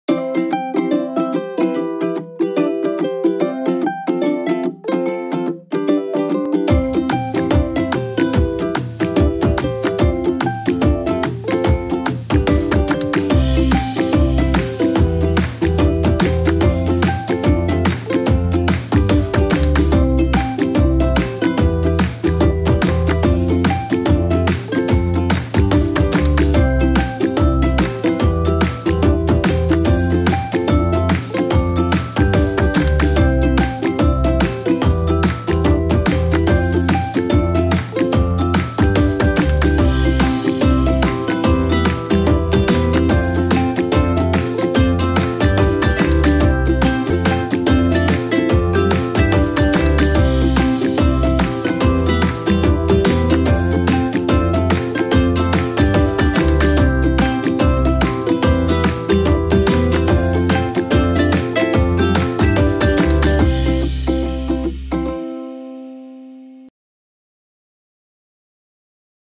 Cranberry Apple Salad Recipe Video
Cranberry Apple Salad is an easy festive side dish perfect for Christmas and Thanksgiving. With crunchy apples, cranberries, arugula, walnuts, and crispy bacon bits, this hearty salad is a sure hit!